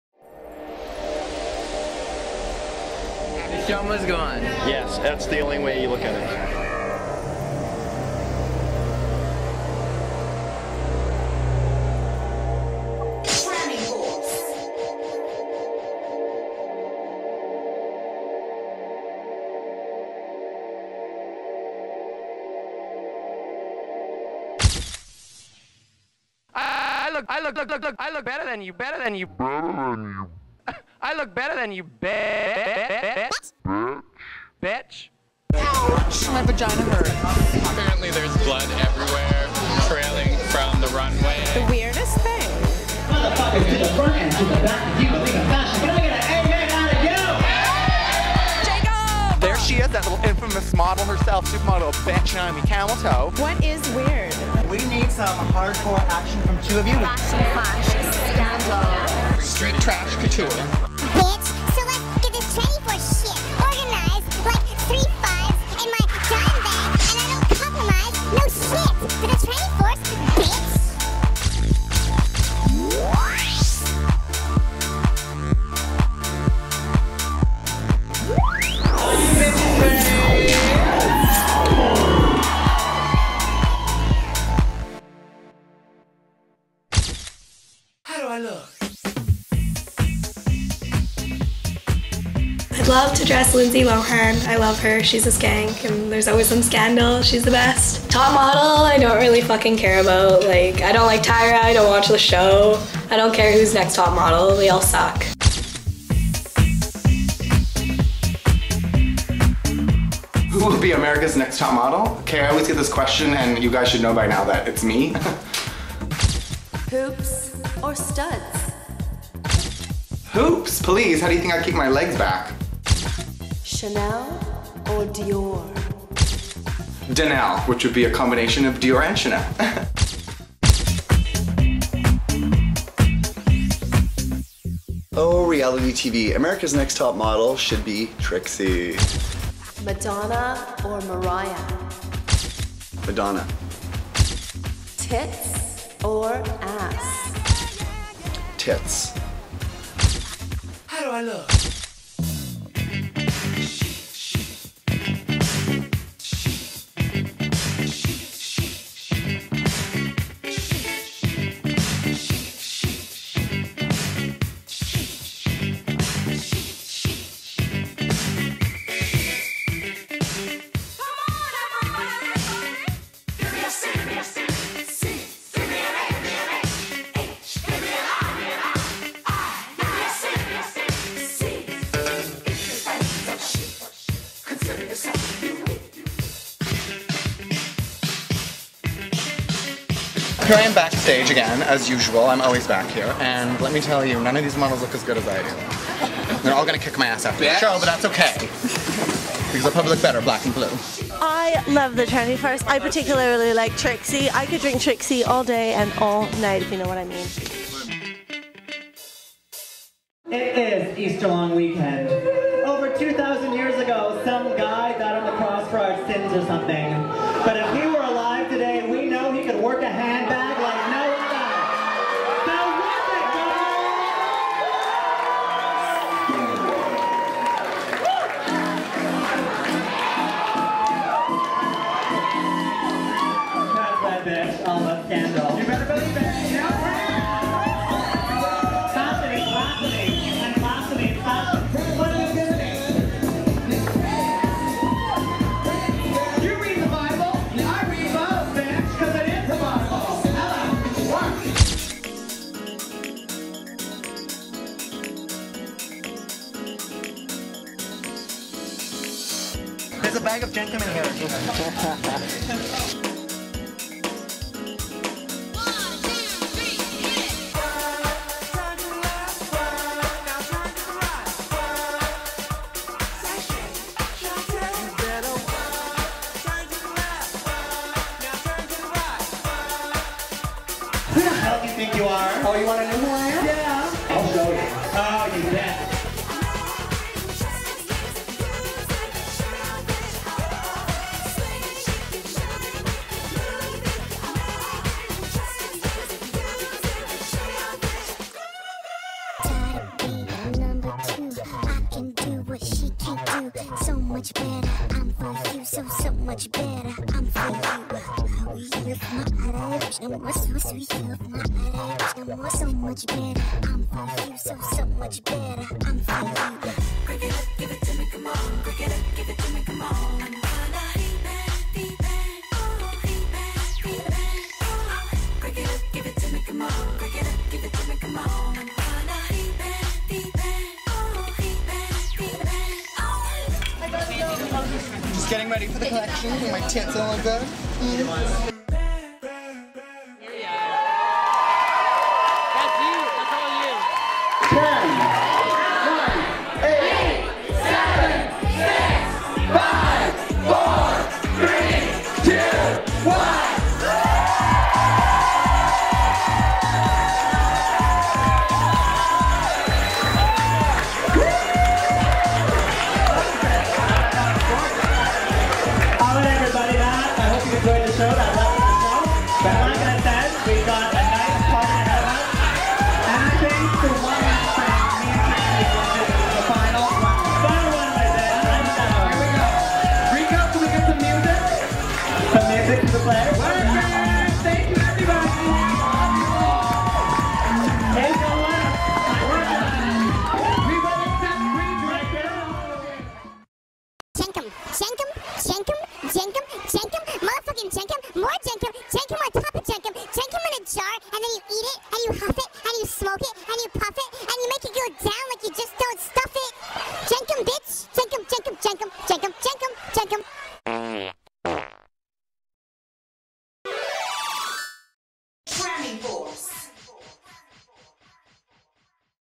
Fashion Flash Live: Fashion Scandal The Show! Haute Couture
Trixie and Milla turned Vancouver into the fashion capital of the world with their haute couture performances and their Vancouver designer friends Justina Anzulovich, Miles Moore and Jason Matlo. Fierce fashions frolicked around as The Tranny Force kicked off spring in style!